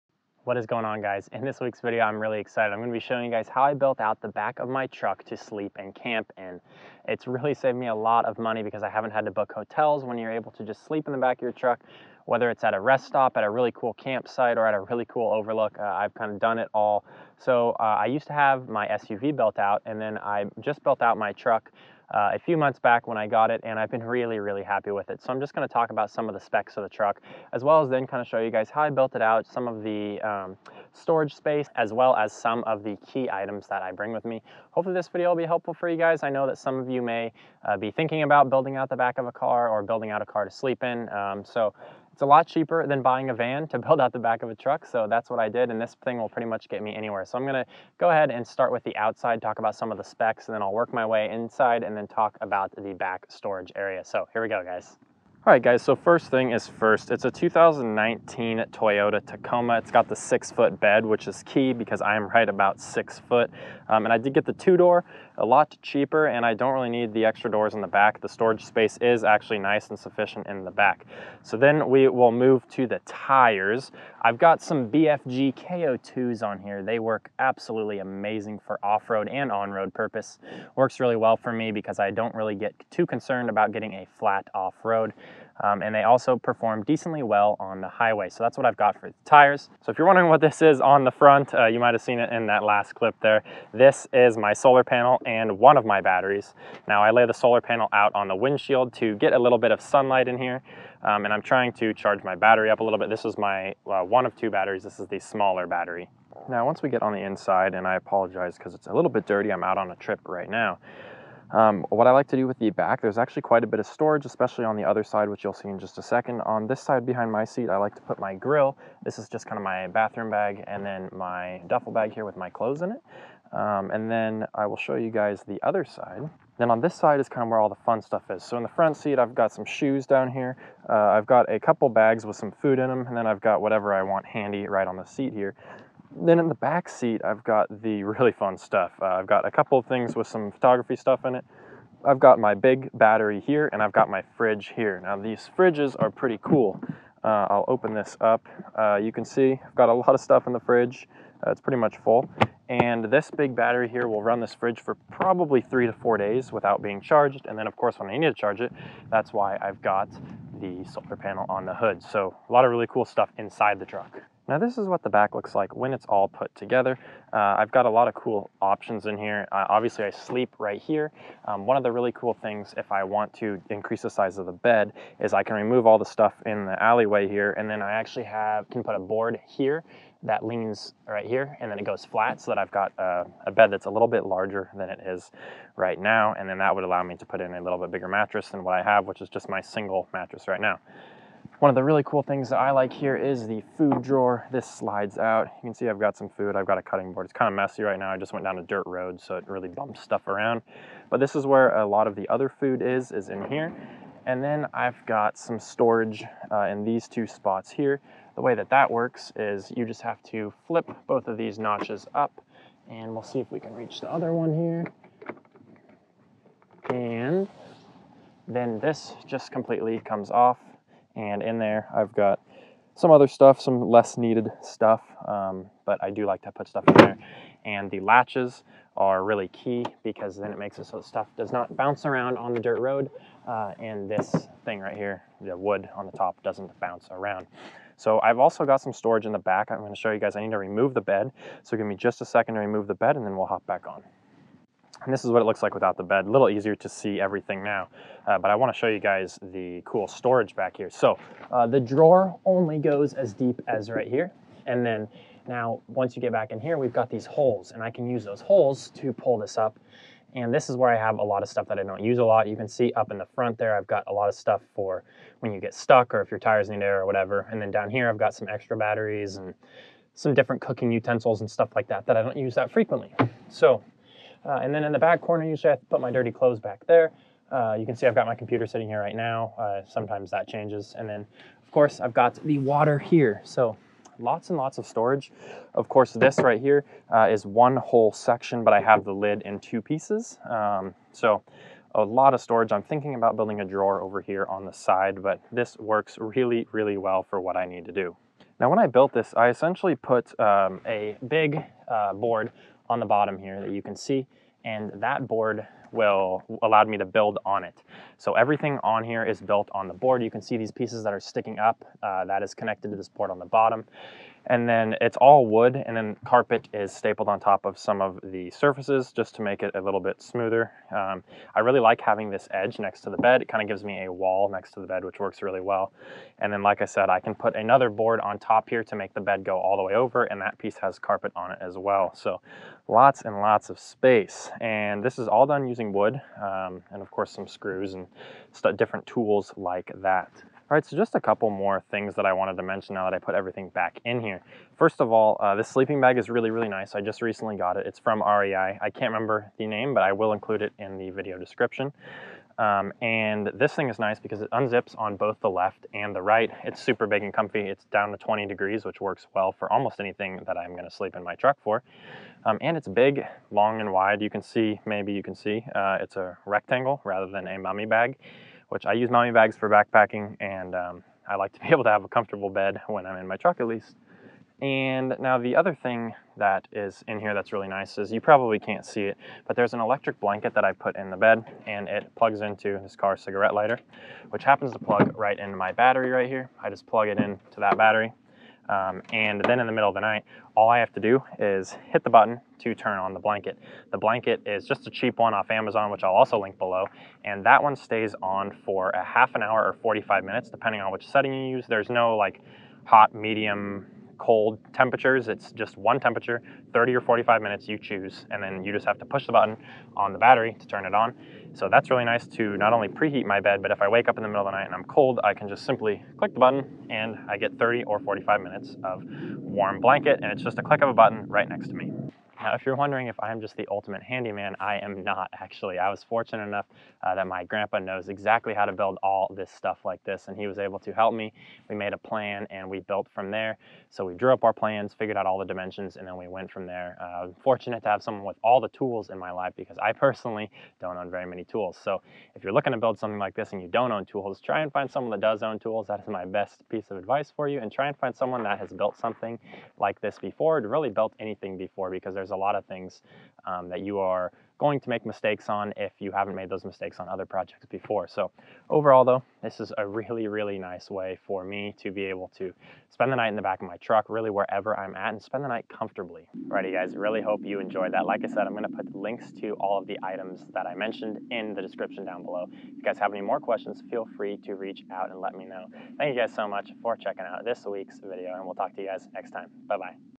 How I Built Out My Truck For Camping and Photography
When I got my truck a few months ago, I quickly built out the back to accommodate my camping and photography travels. Having the ability to sleep in the back of my truck at any point in time is perfect to make sure I am in the right spot at the right time to capture amazing photos. In this video, I show some of the key pieces of gear I use, as well as how the back is built out to sleep in.

Items featured in this video:
Dometic Portable Fridge
Jackery Portable Power Station
Heated Blanket for Car